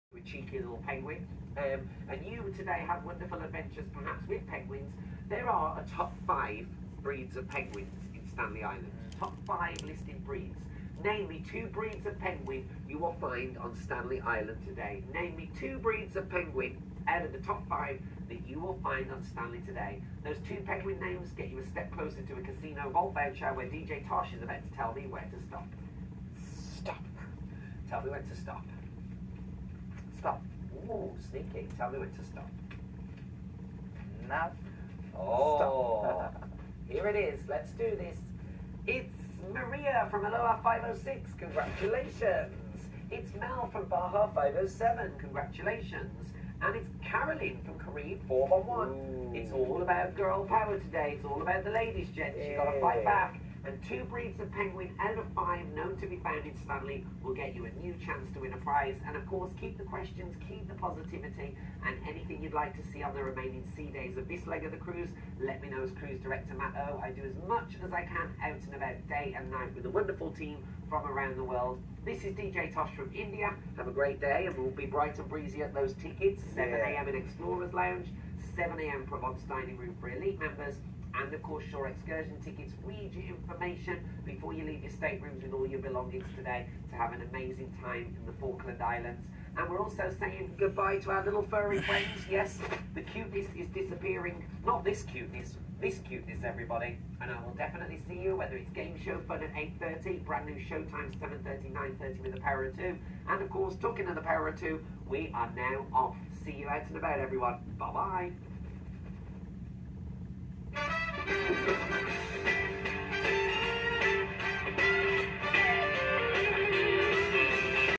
Princess Cruise wake show With Dj Thosh
I was so blessed to part of  Princess Cruises 💕 to be an Assistant Cruise Director and DJ it was a splendid experience. ✨